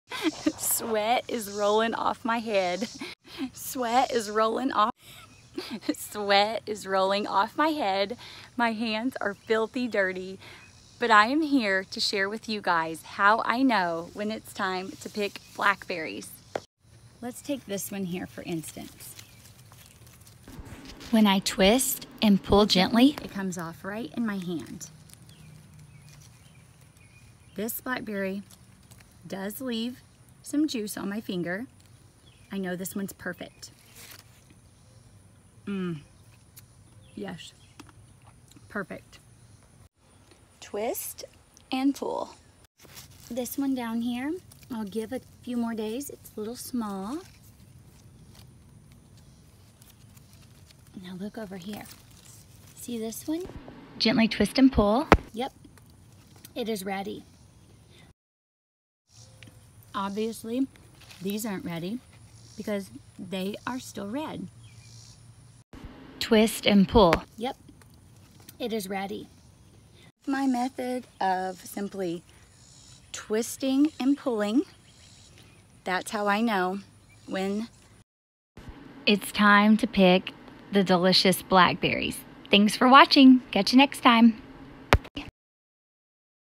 Let’s pick some blackberries!!
It’s a hot day in July-
July 24th to be exact!
I’m picking ripe blackberries in Zone 6b.

Twist & Pull...twist @ pull...that’s my method!

Have a great day! And remember, it’s okay to have sweat rolling down your face while filming a tutorial.  It’s all in the name of helping others!

Don’t take things too seriously and have fun.